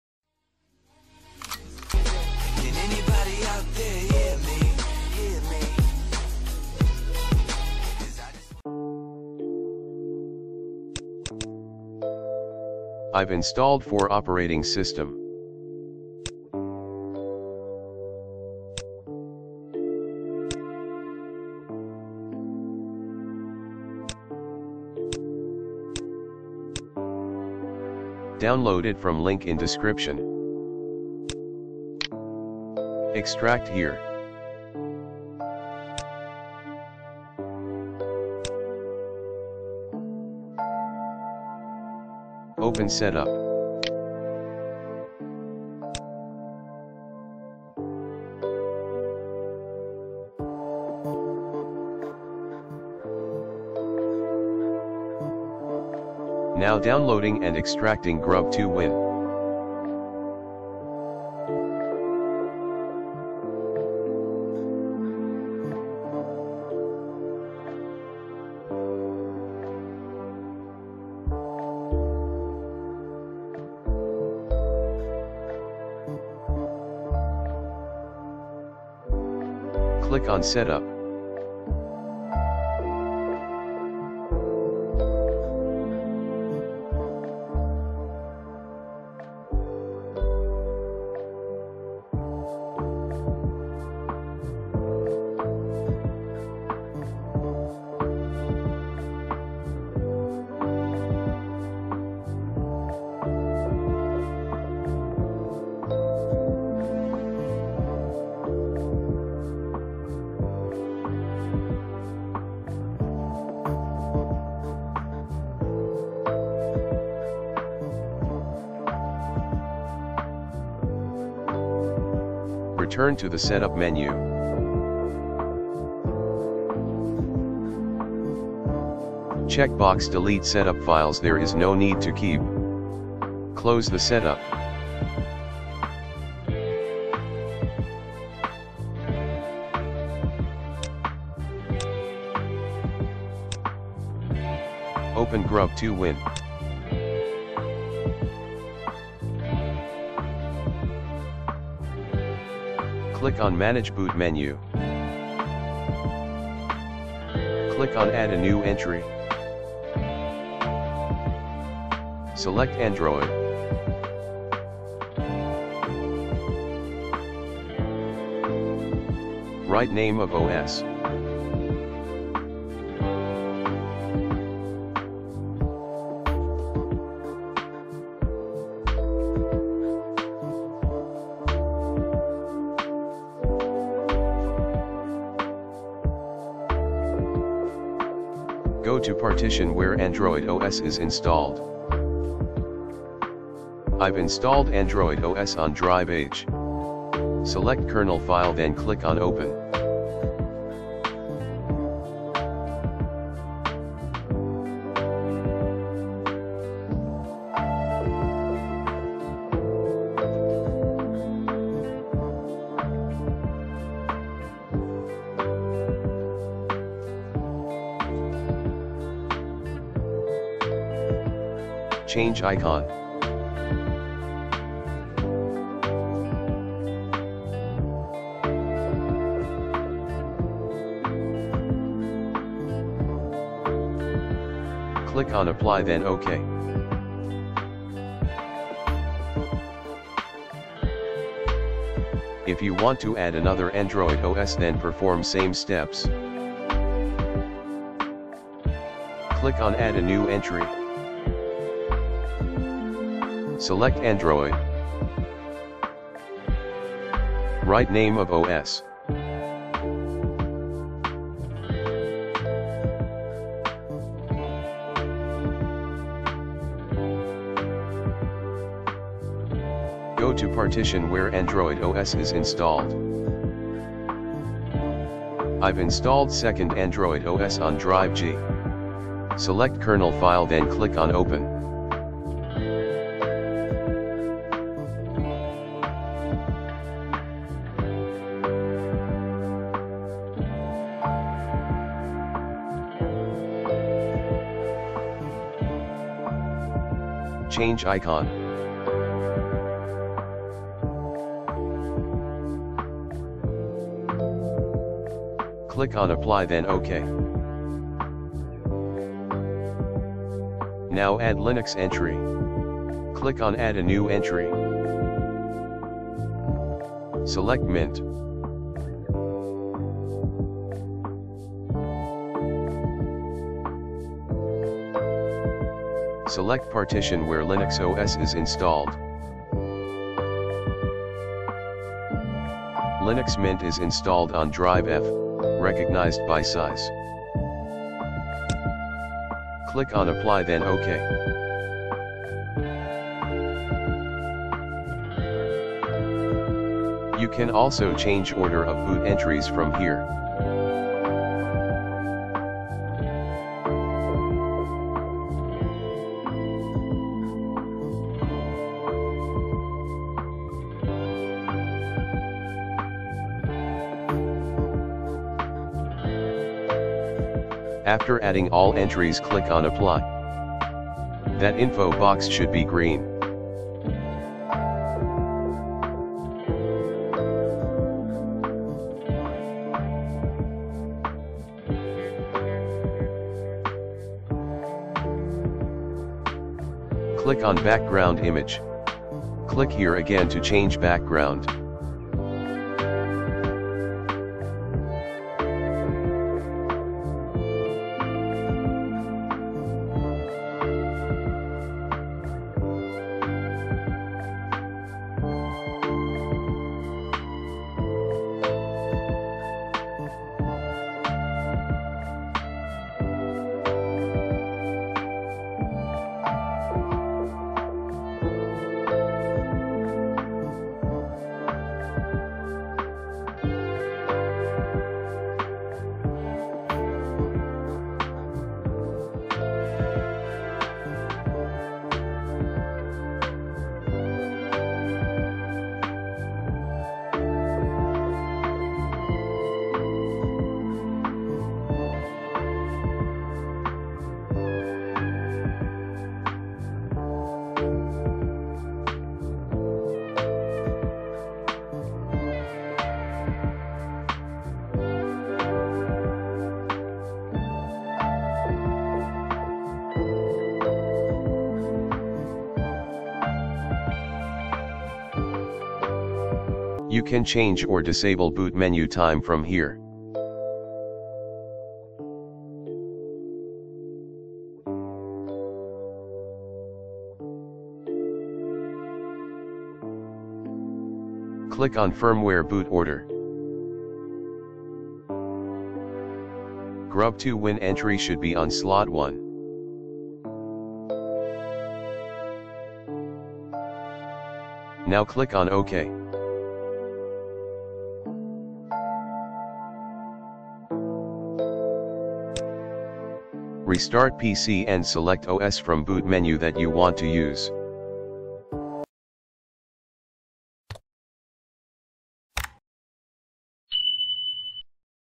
How To Enable Multi Boot Menu In Windows 10 & 11 |Show OS Selection Menu | Windows + Linux + Android
Welcome To Gemini 360

Hi Viewers

In This Video I'll Show How To Boot Android & Linux Operating System Along with Windows By Using Grub2Win Software.

What Is Grub2Win?
Grub2win Is A Free Tool That Allow To Add Multiple Boot Entries For Different Operating System
It Helps To Enable Multi Boot Menu In Windows By Easy Method.
Works with both EFI and BIOS firmware.
Supports Windows 11, 10, 8, 7 & XP

Comment Language:
 English
 Urdu